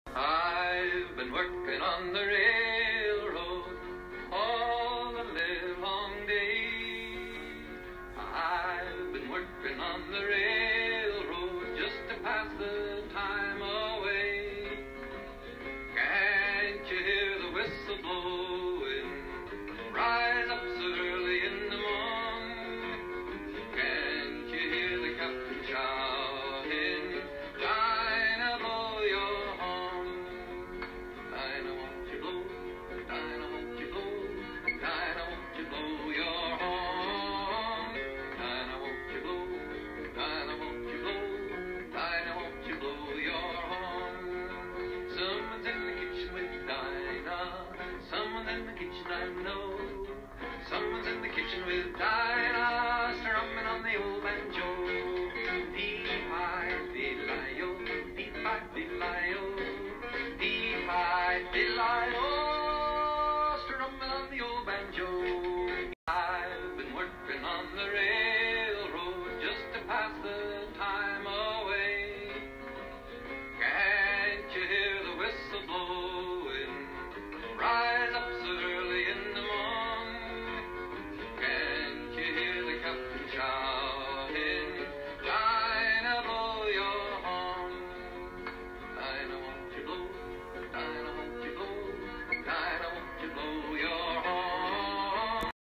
Portuguese trains part2🚝🛤🇵🇹
Some of the different types of trains you can see in Porto Portugal freight trains and various passenger trains if you’ve enjoyed this video I have another video of Portuguese trains on my channel